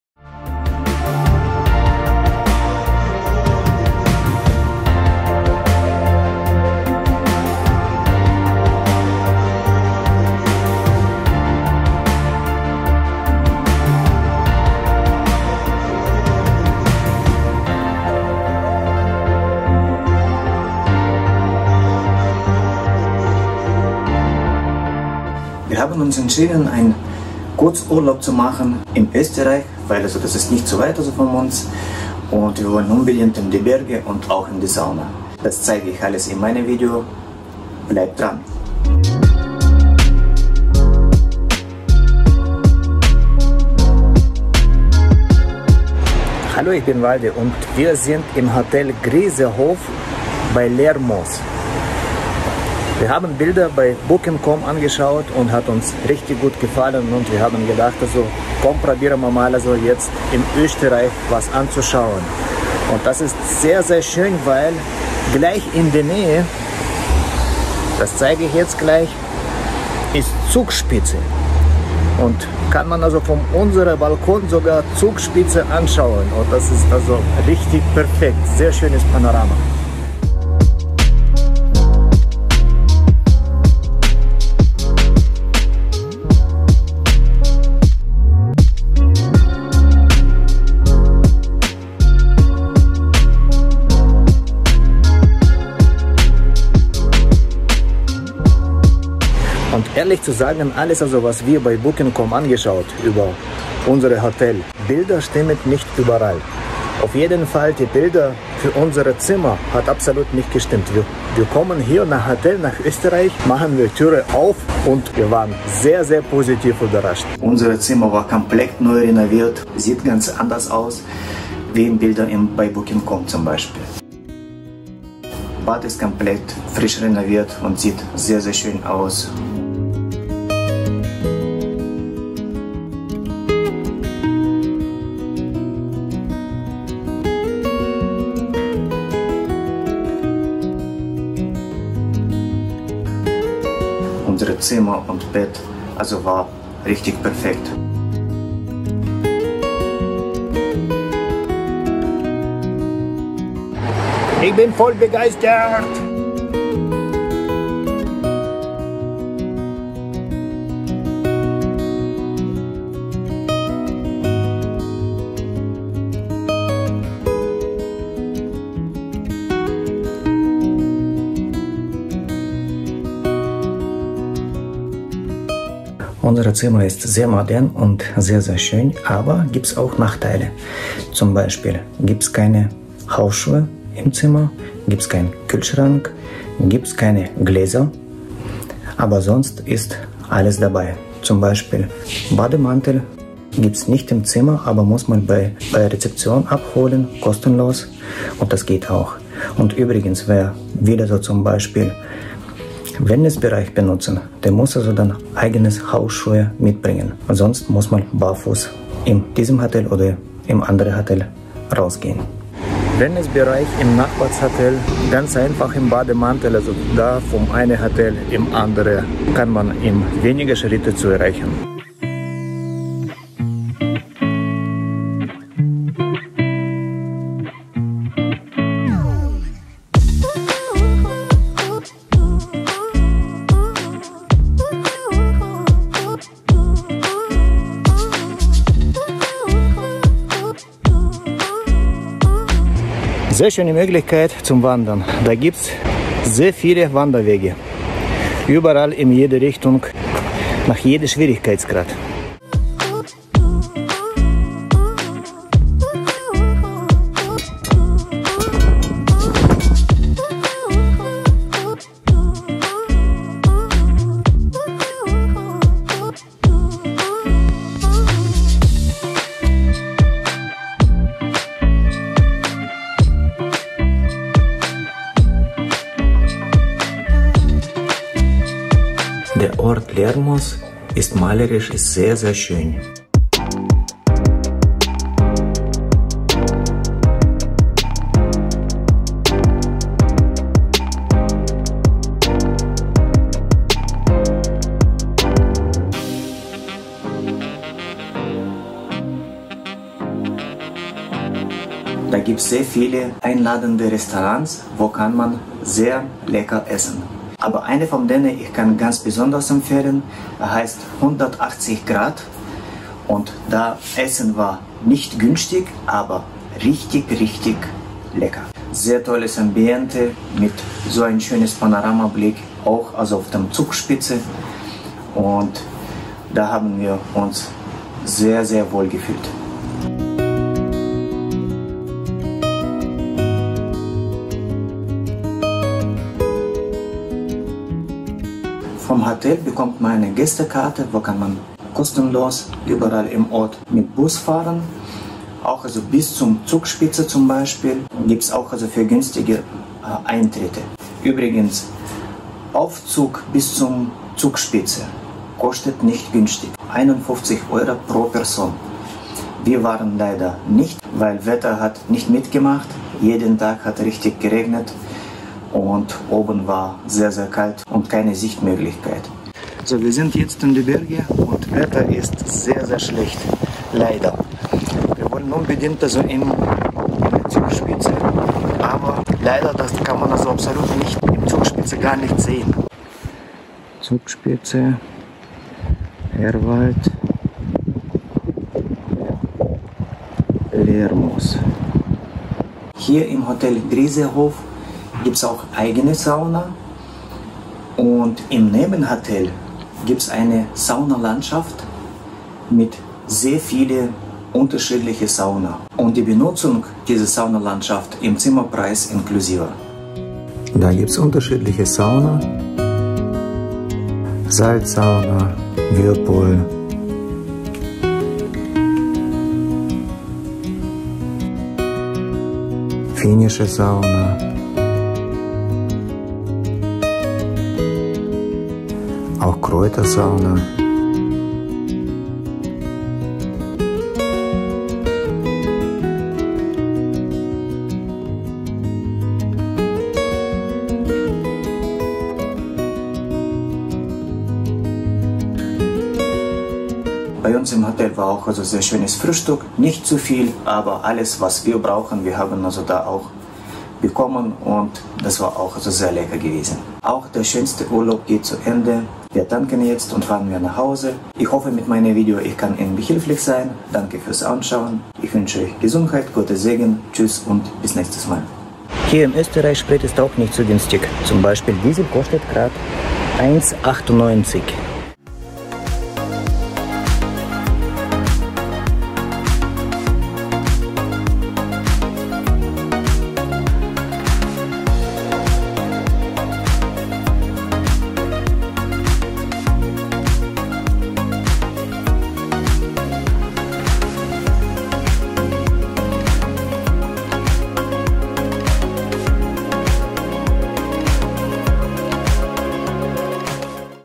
Wellnessurlaub in Tirol: Hotel Griserhof in Lermoos – Entspannung in den Alpen
Wellnessurlaub in Tirol – Unser spontaner Aufenthalt im Hotel Grieserhof, Lermoos
In diesem Video nehme ich euch mit auf unsere spontane Reise nach Lermoos und erzähle von unserem unvergesslichen Wellnessurlaub im wunderschönen Hotel Grieserhof. Da es unser erster Besuch in dieser traumhaften Region war, wussten wir nicht, was uns erwartet – und wurden rundum positiv überrascht!

Was erwartet euch im Video?
 • Erste Eindrücke: Unser erster Blick auf das Hotel und die Umgebung
 • Wellness & Entspannung: Eindrücke von den Wellnessbereichen und den besonderen Annehmlichkeiten des Hotels
 • Alpen-Feeling: Die beeindruckende Landschaft Tirols und der Charme von Lermoos
 • Persönliche Tipps: Unsere Highlights und Inspirationen für euren nächsten Urlaub

Lasst euch von der herzlichen Atmosphäre und der atemberaubenden Natur verzaubern – vielleicht ist das auch genau das, was ihr für euren nächsten Urlaub sucht!

Unterstütze mich:
📙 Mein Buch